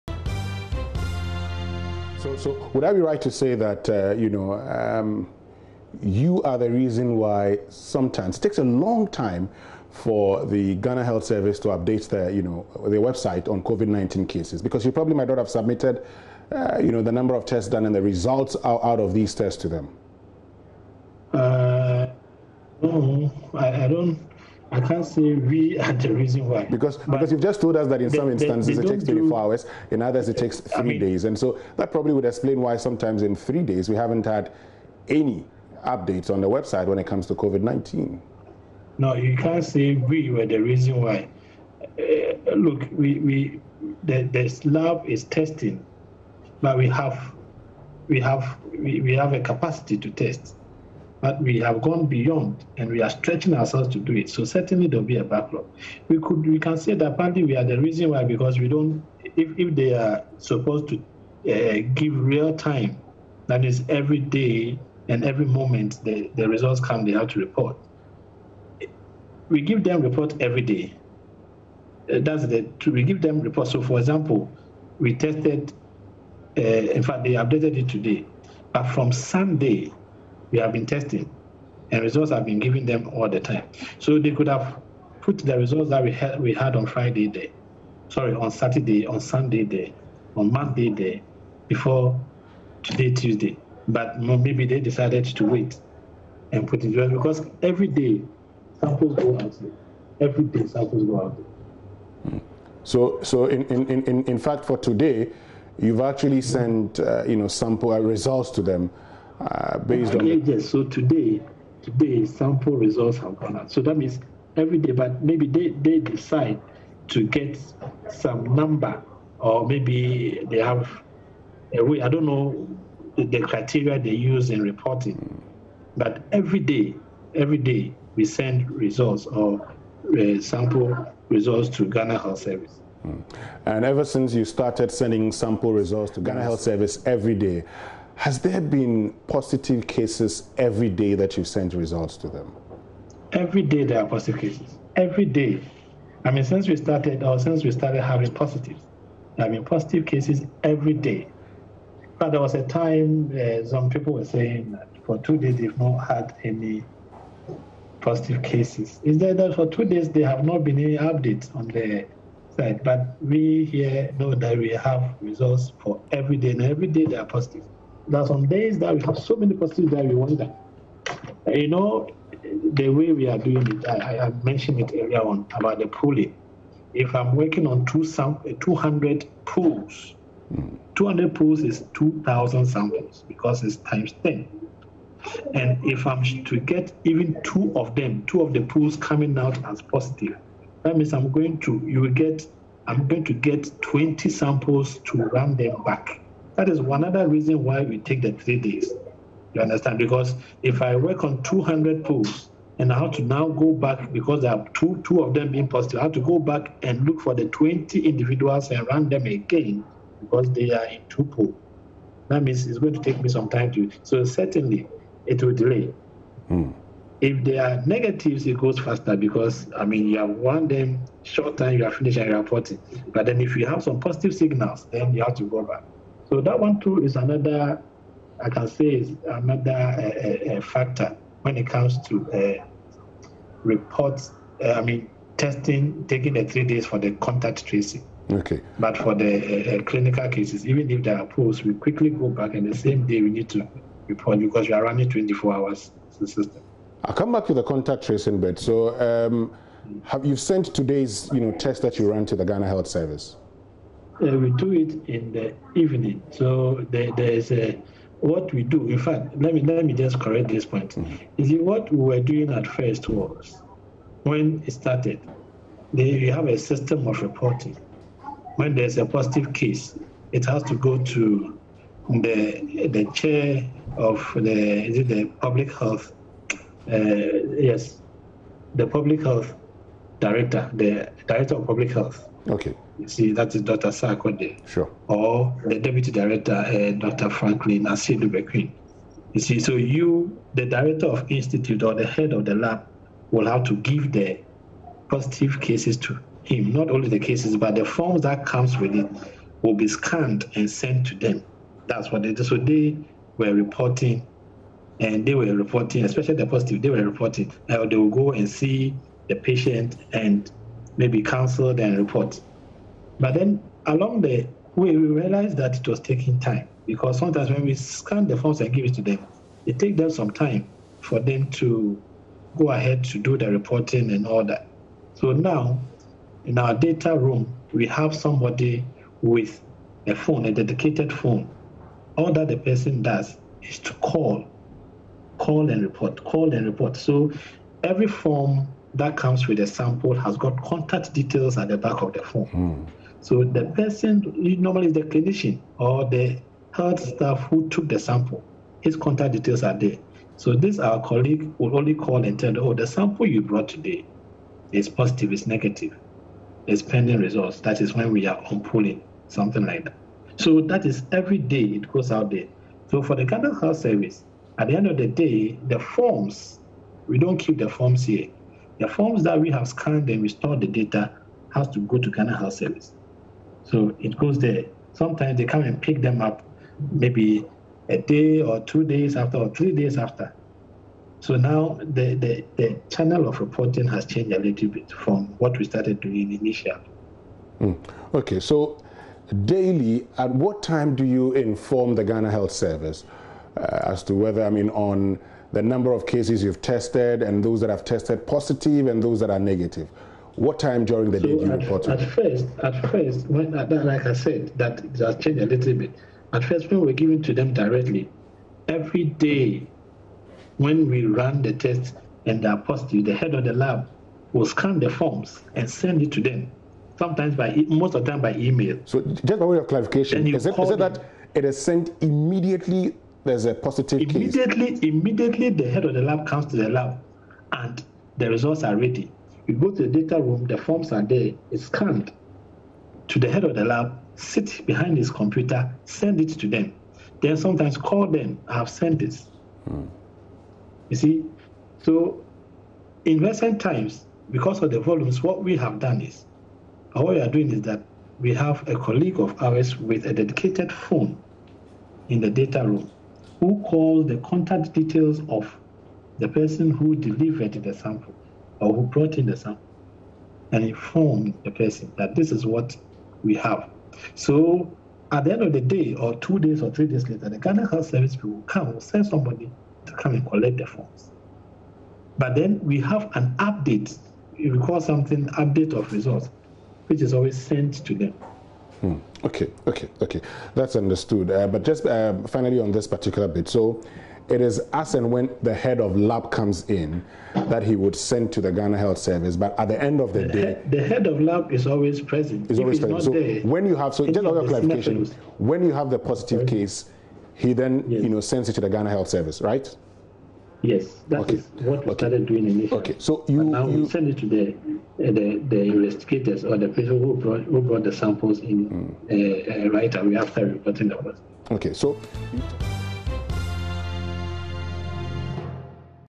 Noguchi: We provide Ghana Health Service with test result daily - Joy News (22-4-20)
Noguchi: We provide Ghana Health Service with test result daily